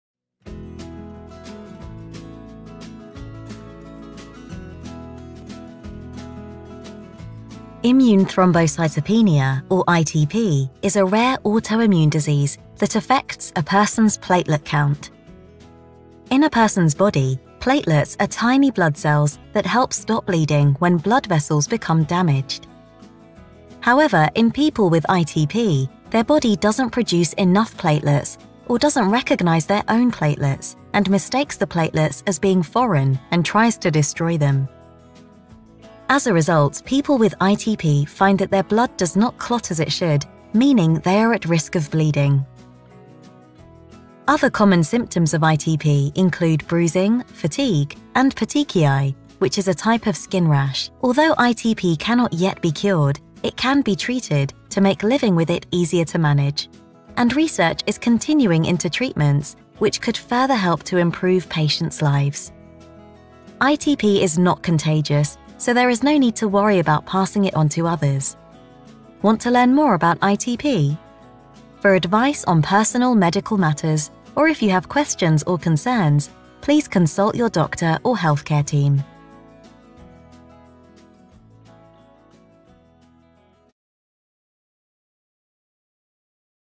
Live Life with ITP - What is ITP
Immune thrombocytopenia or ITP, is a rare
autoimmune disease that affects a person’s platelet count.